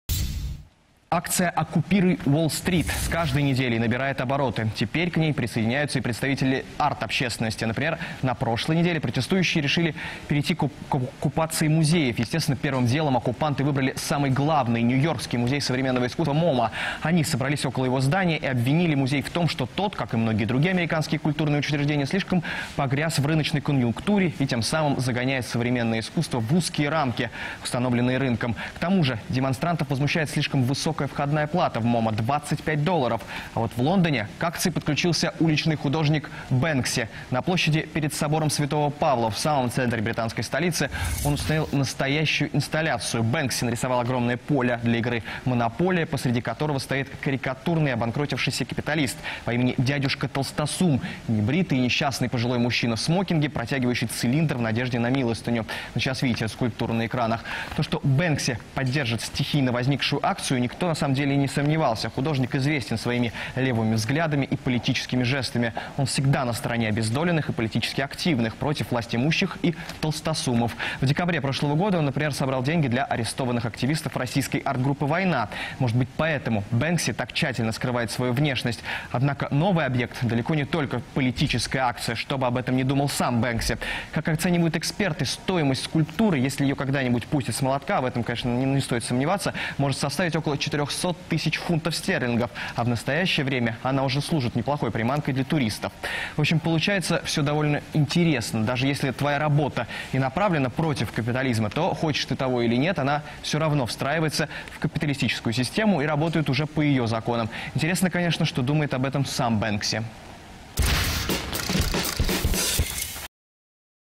Бэнкси оккупирует Уолл-Стрит
Художник Бэнкси присоединился к акции "Оккупируй Уолл-Стрит"